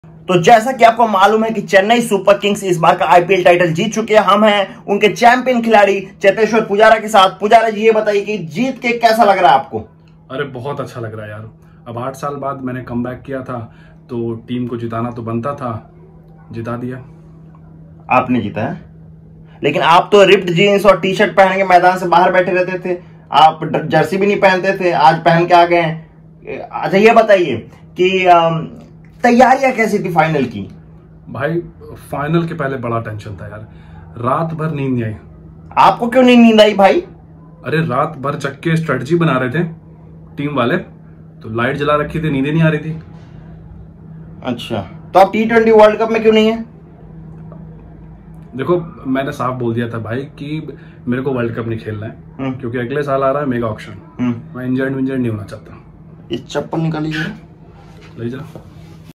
Pujara after CSK wins IPL Satish Ray | Mr Ray
Pujara taking all the credit. Made only for fun. We all are his fans.